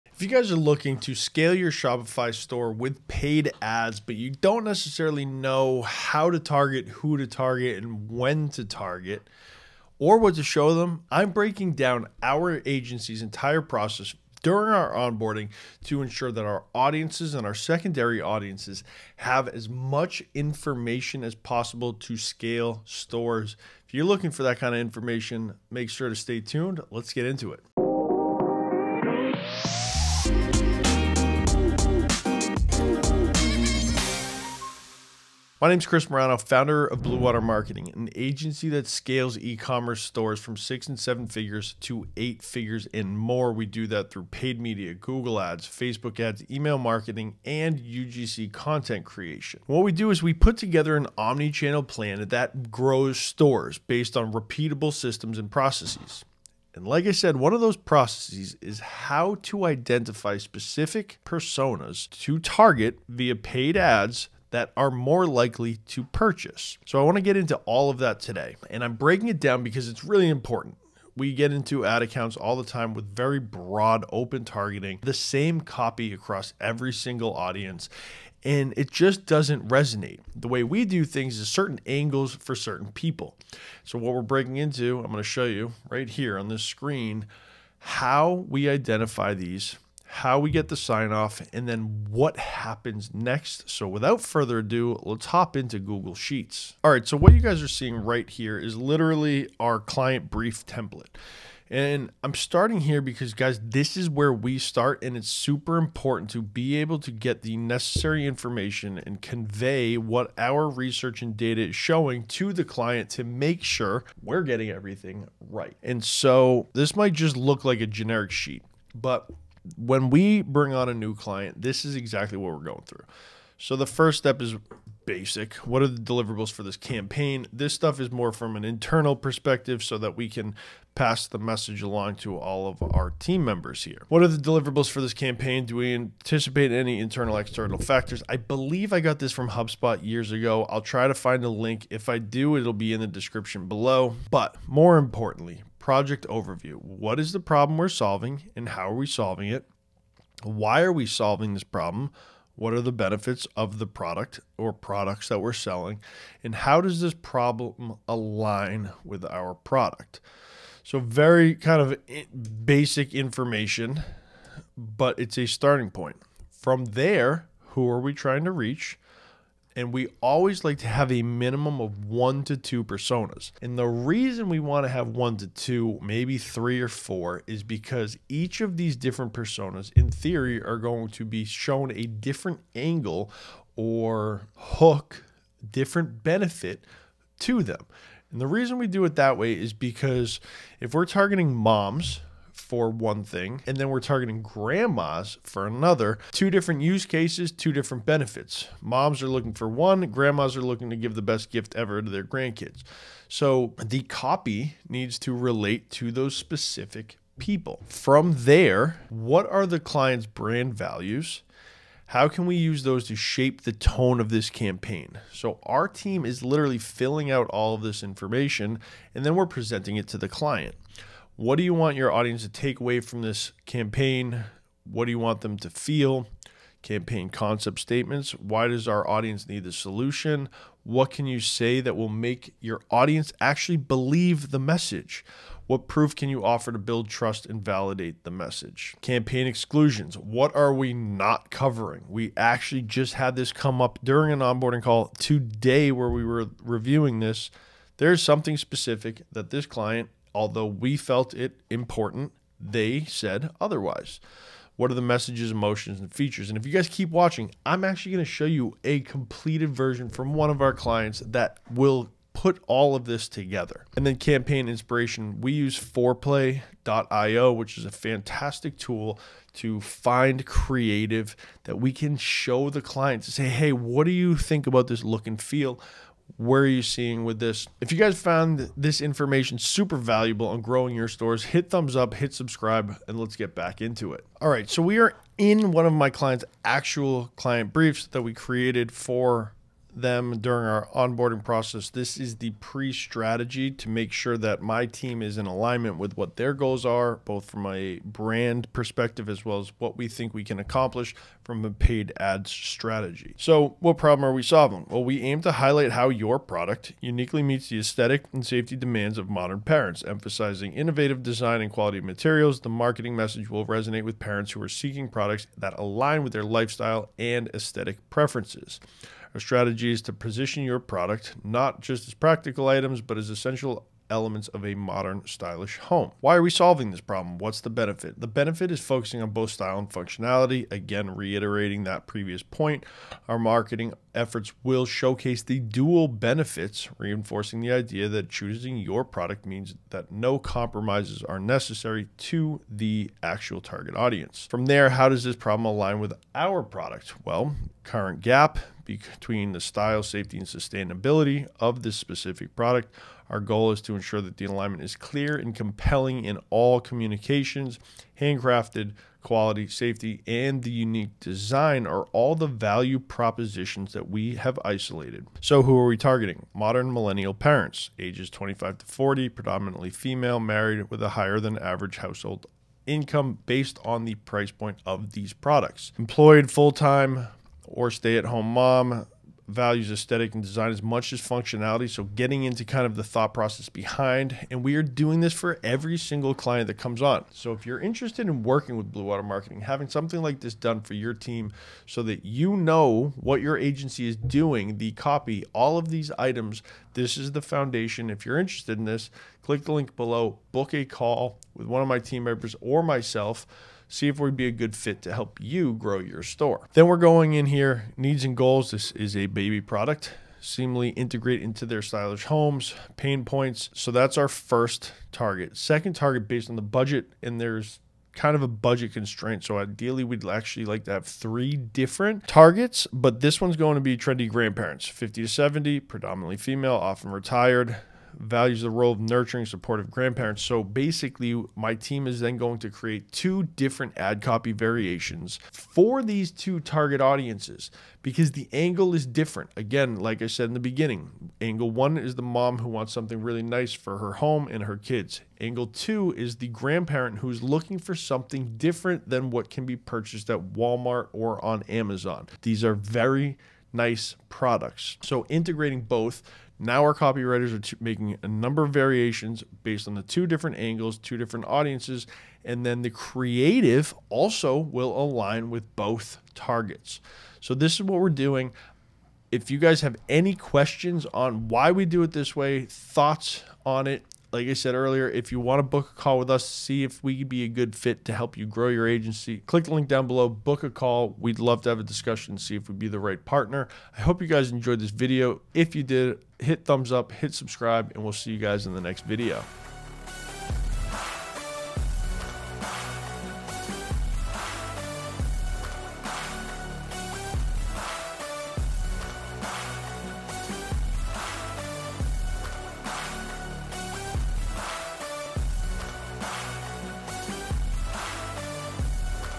Scale Your E-Commerce Store with Our Secret Framework!
Ready to scale your e-commerce store to new heights? 🚀 In this video, I break apart our framework that has successfully scaled online stores from six and seven figures to eight figures and beyond! Dive into our no-nonsense guide on how to leverage paid ads, Google Ads, Facebook Ads, email marketing, and UGC content creation to skyrocket your sales.

We start by breaking down the onboarding process, where we analyze and refine audience targeting. You'll learn how to identify specific personas that will most likely convert, ensuring your ads resonate perfectly with each segment. I'll walk you through our client brief template on Google Sheets, showing you exactly how we gather and utilize crucial information to craft tailored campaigns.

Understand the importance of an omni-channel approach and how different angles work for different people. Whether you're targeting modern millennial parents or trendy grandparents, we strategize to meet diverse needs and preferences. This ensures that your campaign's messaging aligns seamlessly with your brand values, enhancing customer connection and trust.

By the end of this video, you'll be equipped with actionable insights and a clear understanding of how to implement these strategies in your own business. Don't miss out on learning from an agency that knows how to make the magic happen!

Resources

CHAPTERS:
0:00 - Introduction
1:36 - Client Brief Template
4:44 - Client Brief Example